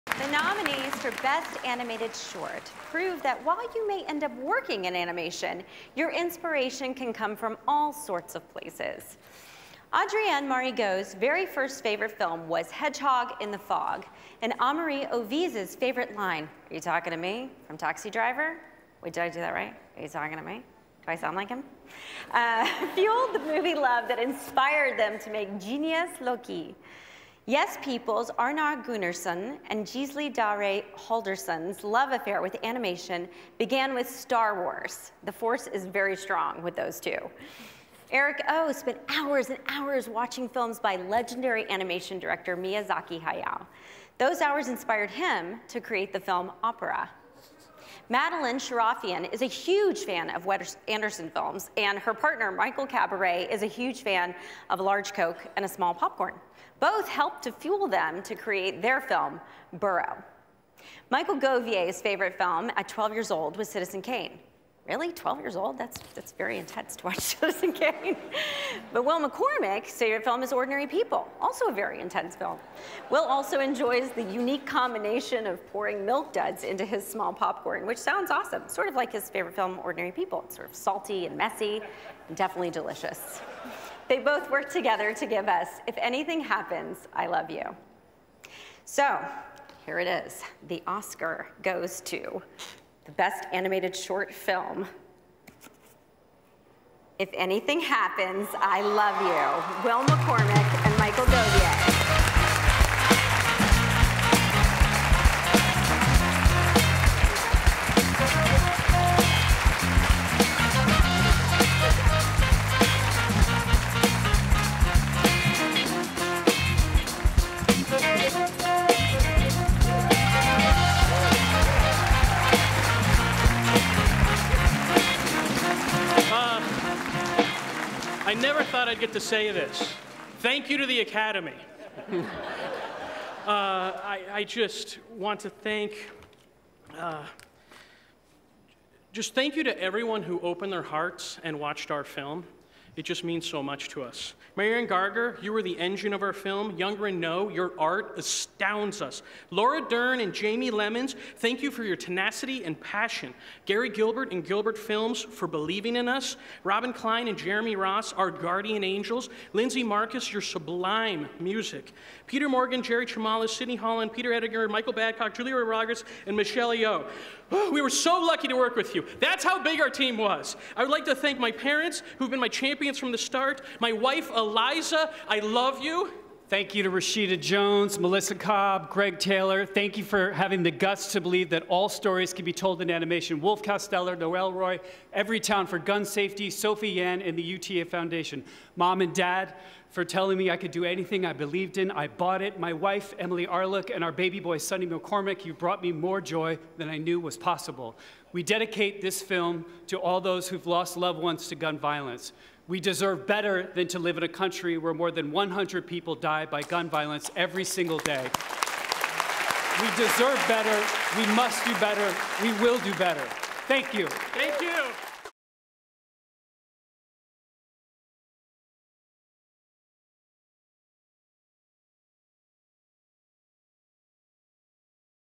"If Anything Happens I Love You" Wins Best Animated Short Film | 93rd Oscars
Reese Witherspoon presents Will McCormack and Michael Govier with the Oscar for Best Animated Short Film for "If Anything Happens I Love You" at the 93rd Oscars.